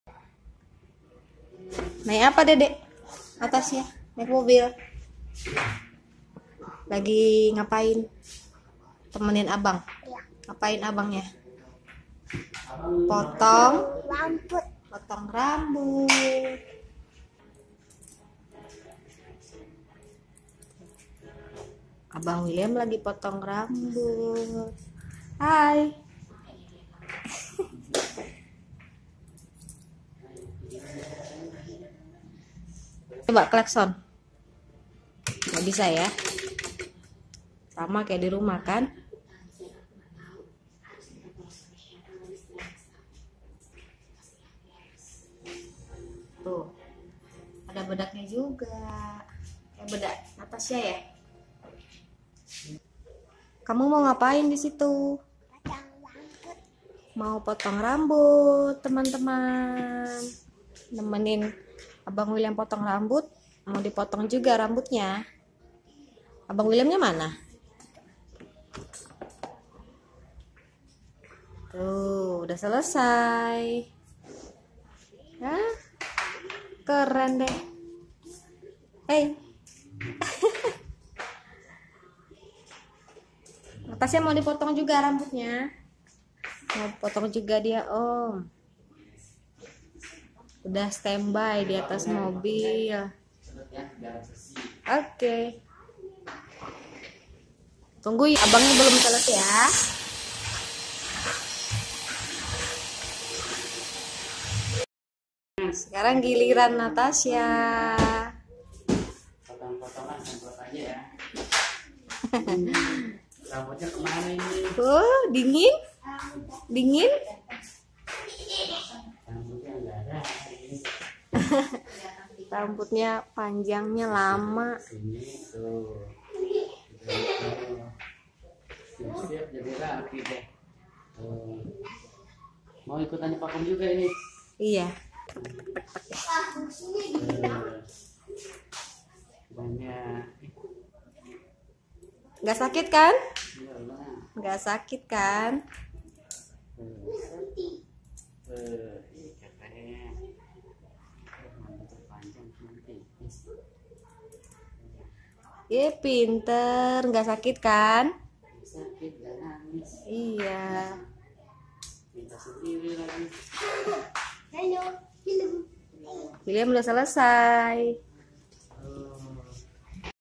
Natasha Temenin Abang William Potong Rambut Jadi Ikutan Potong Rambut -- Baby Hair Cut
Natasha awalnya cuma temenin abang William potong rambut nih temen2, eh malah jadi ikutan potong rambut juga dia! Soalnya dia betah krn ada mobilannya sebagai tempat duduk saat di potong rambutnya. Hehehe..